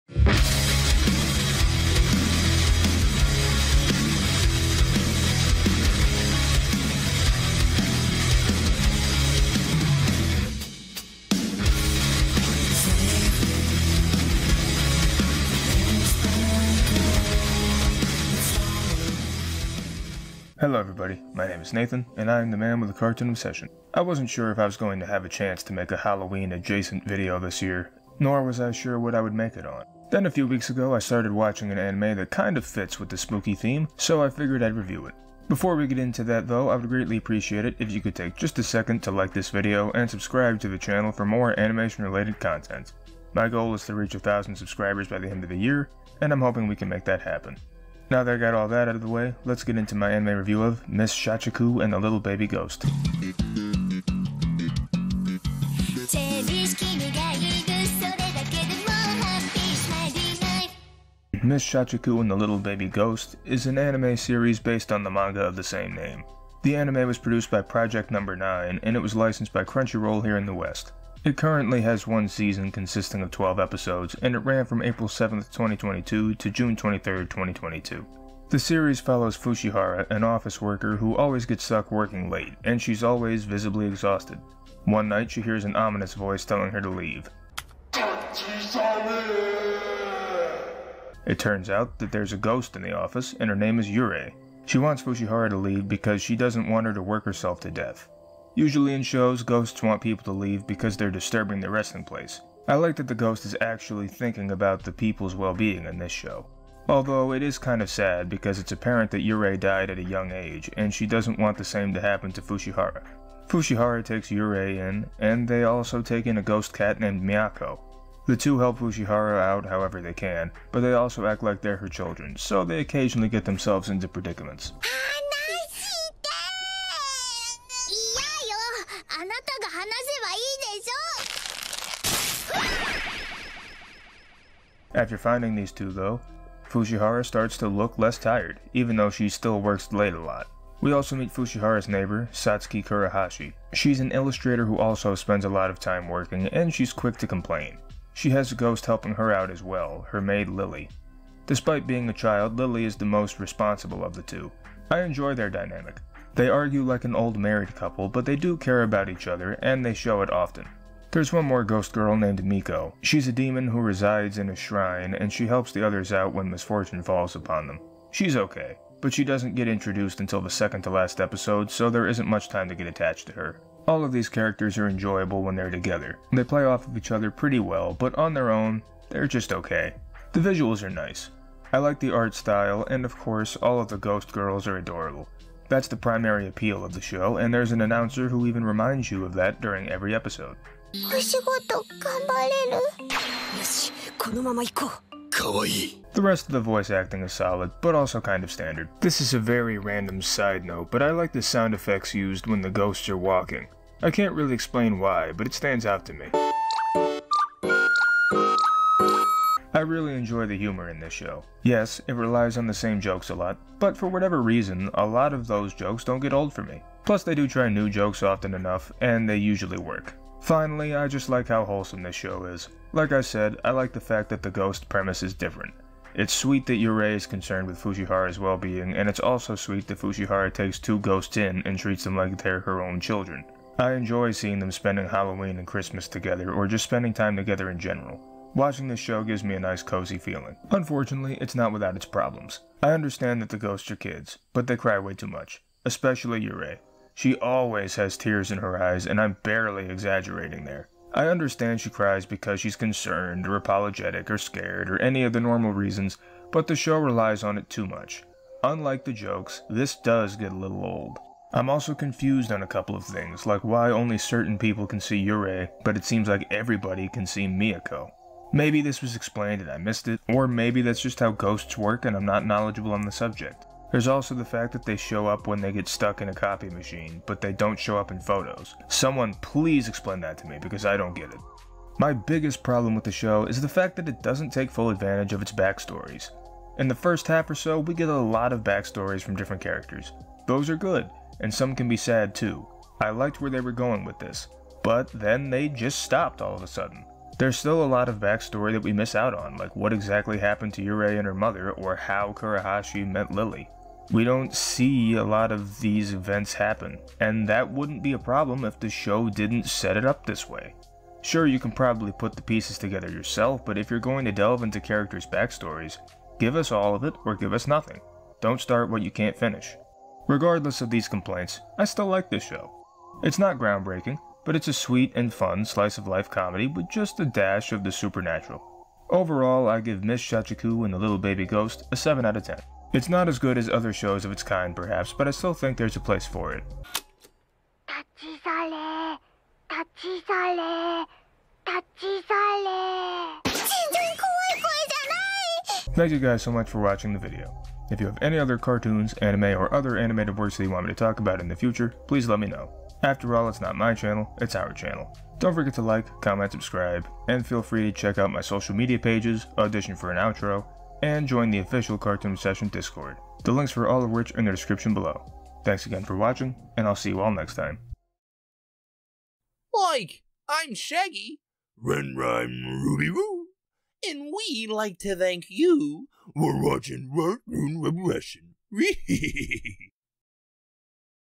ANIME REVIEW: Miss Shachiku and the Little Baby Ghost
To celebrate the spooky season, I will be reviewing the anime: Miss Shachiku and the Little baby Ghost.